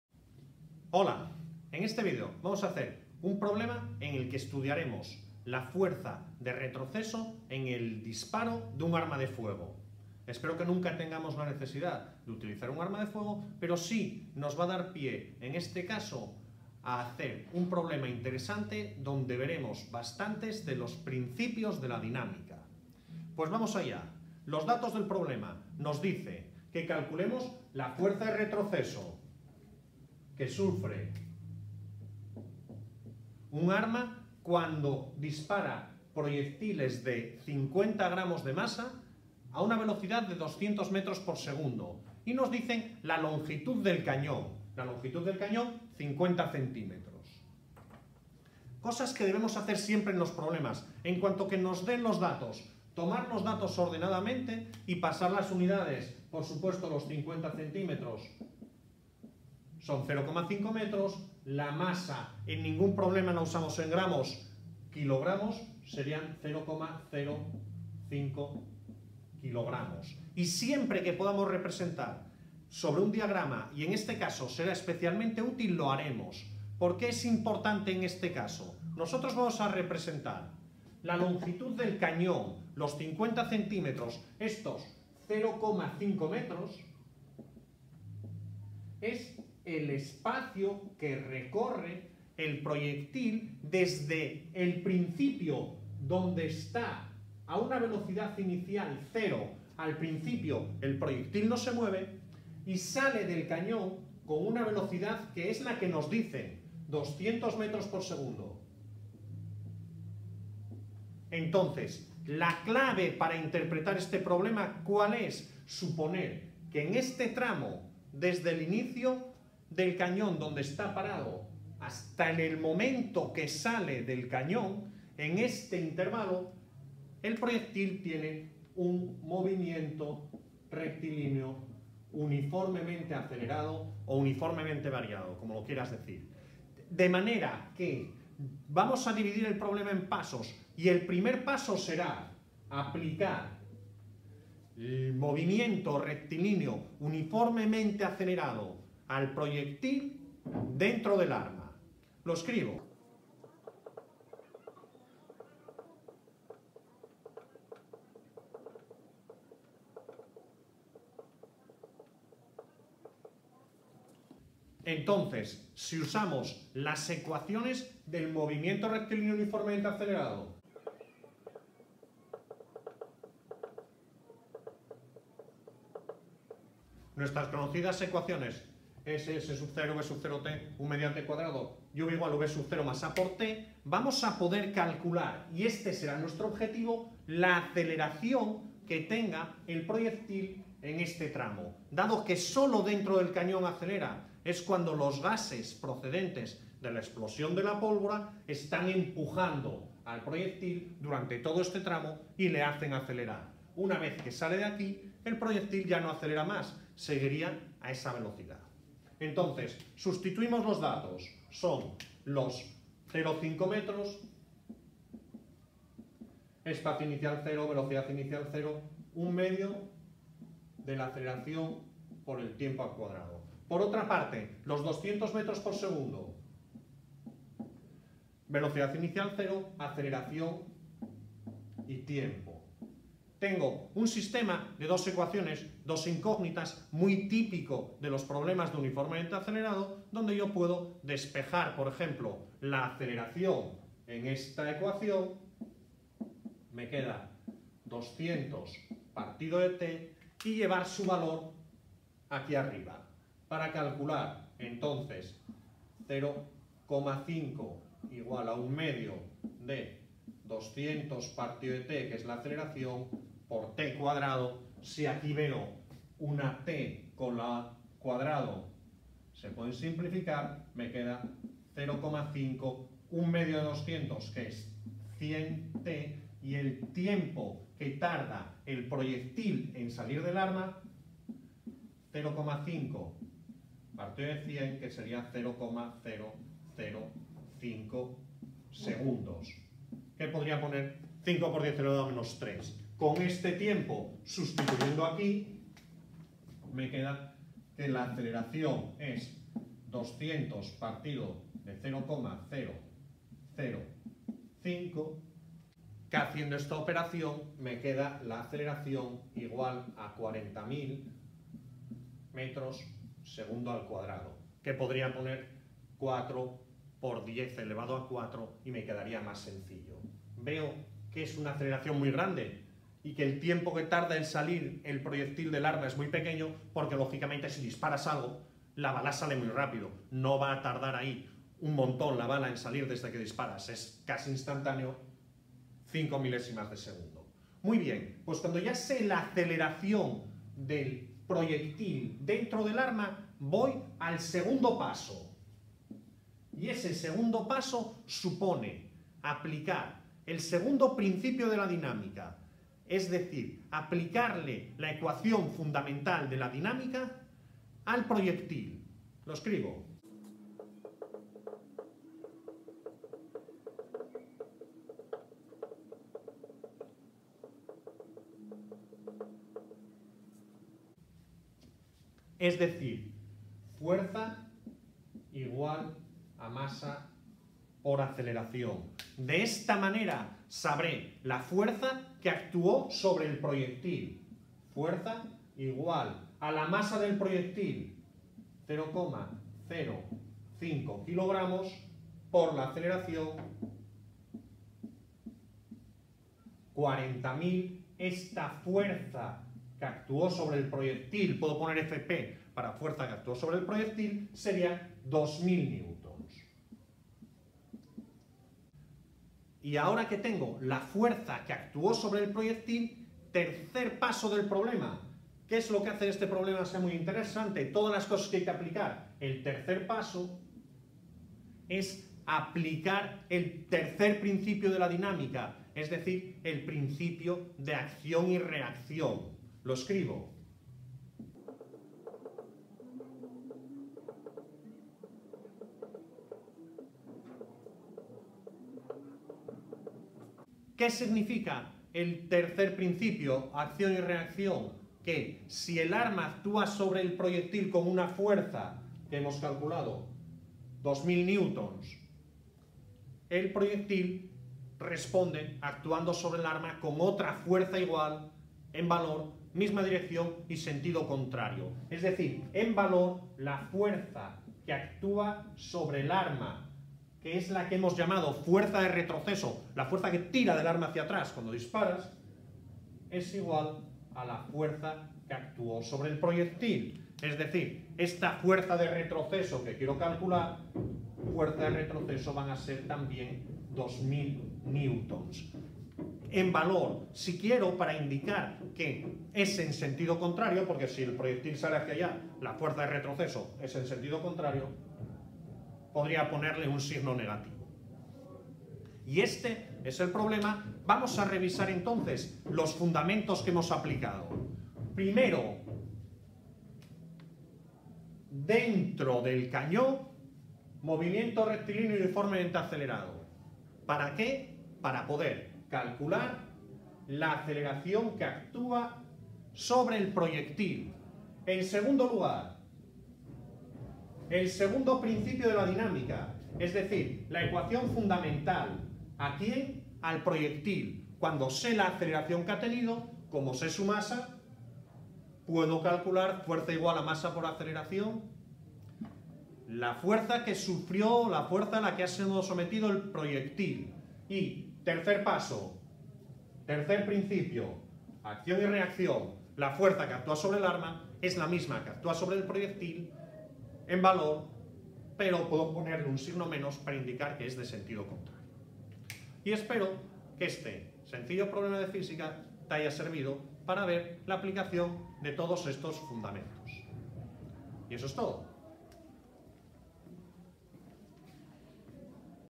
Fuerza de Retroceso. Principios de la dinámica
Calcular la fuerza de retroceso de un arma de fuego por aplicación de los principios de la dinámica y el movimiento MRUA. Problema clásico de física explicado paso a paso